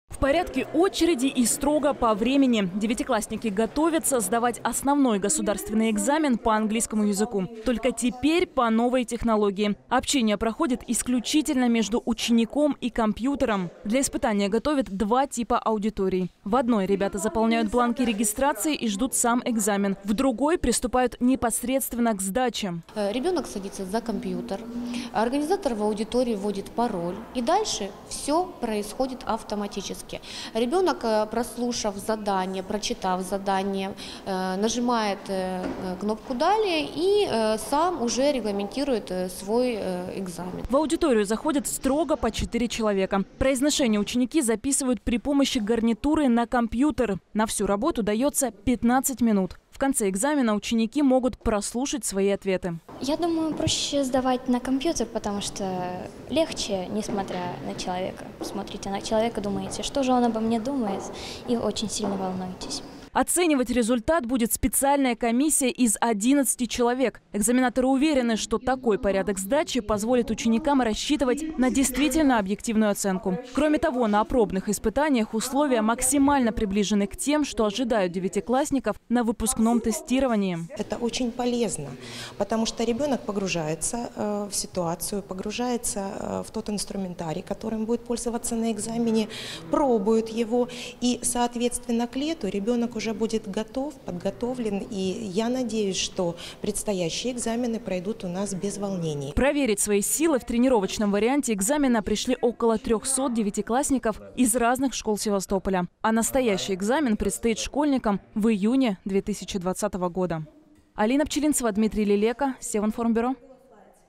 Севастопольские школьники готовятся к ОГЭ по английскому языку
Около трёхсот девятиклассников севастопольских школ готовятся к сдаче основного государственного экзамена по английскому языку. С этого года для учащихся всей России вводят новую технологию: раздел «Говорение» ребята будут сдавать только при помощи компьютера. Тренировочное испытание для севастопольцев прошло 19 ноября в гимназии №7 имени Виктора Великого.